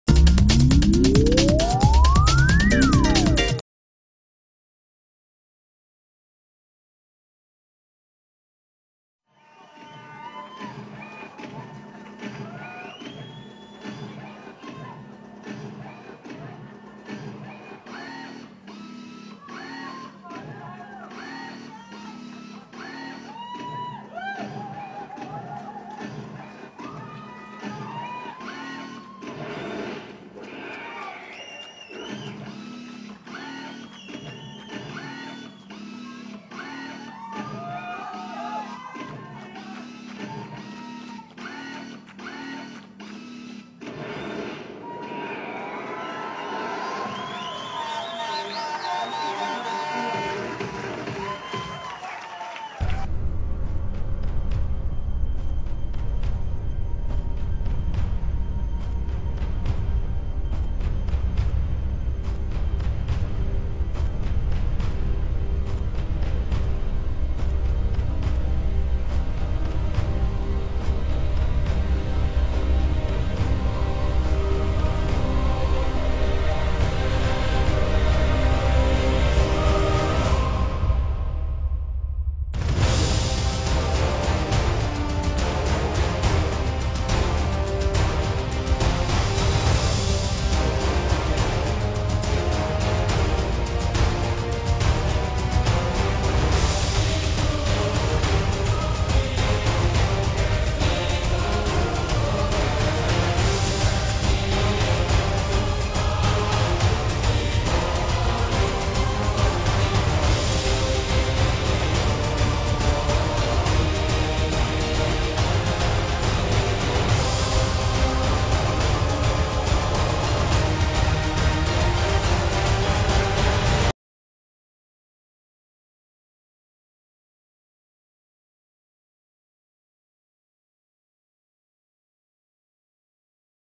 NABBA Britain 2009 - Part 2
NABBA Britain 2009 Body Building Championships, part 2. The final part of one of the biggest body building events in the UK, NABBA British Finals.